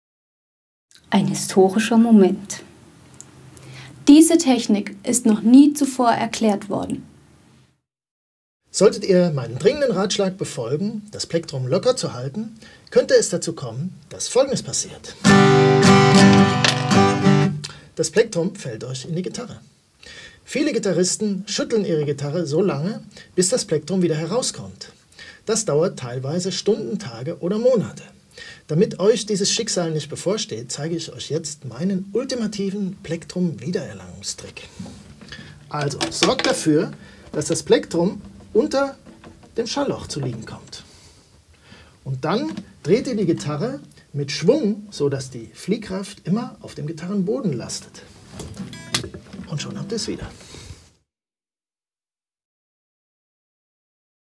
Das Plektrum Wiedererlangen - Video aus Peter Autschbach "Rock On Wood"
Das Video ist eines der Videos meines Akustikgitarren-Lehrbuchs "Rock On Wood".
Beim Strumming klingt es wesentlich besser, wenn man das Plektrum locker hält. Bei Beachtung dieses spieltechnischen Tipps kommt es gerade am Anfang häufig vor, dass das Plektrum in den Korpus der Gitarre fällt. Hier seht ihr, wie man es auf einfache Weise zurückbekommt, ohne stundenlang die Gitarre zu schütteln.